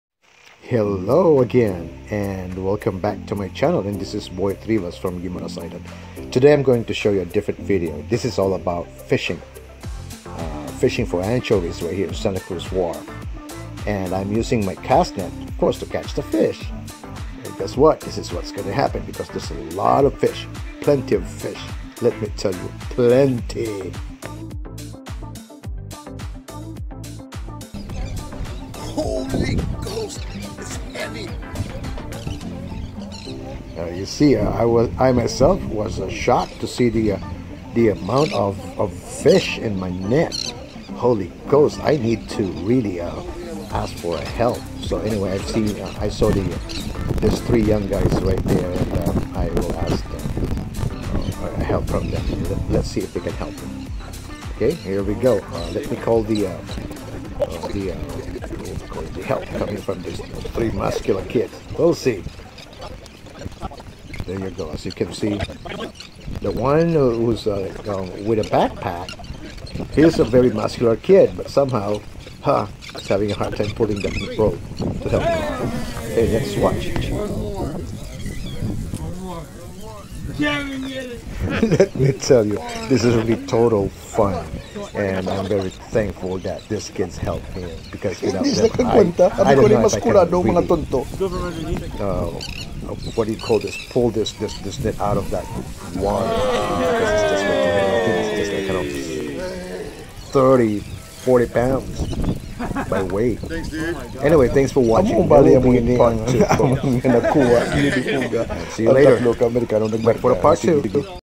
Dilis na Isda using Lambat -Santa Cruz Wharf Fishing Aug 2020 .. Anchovies are here to stay
Dilis o Balingon fish para e consumo sa entero nga tu-ig
Ang tamanio sang atun net? 6 pies Mesh un cuarto 1/4
Naga balor cien dolares ang Net. Good investment after all..